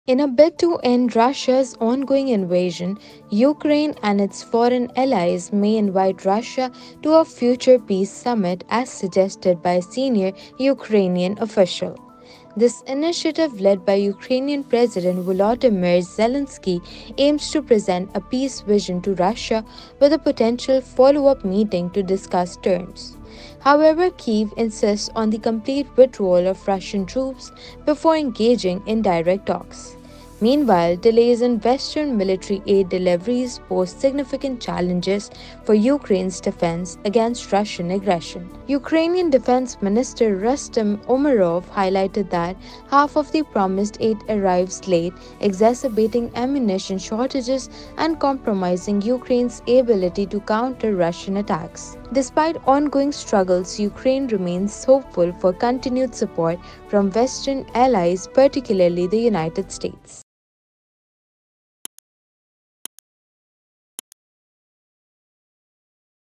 Ukraine Floats Possibility Of Inviting Russia To Summit | Dawn News English
Ukraine and its foreign partners could invite Russia to a future peace summit to discuss an end to Moscow’s two-year-old invasion on Kyiv’s terms, a senior Ukrainian official said on Sunday.

Switzerland will host a summit to discuss a vision for peace by Ukrainian President Volodymyr Zelensky, which could be handed to Russia during a second meeting at a later date, said Zelensky’s chief of staff Andriy Yermak.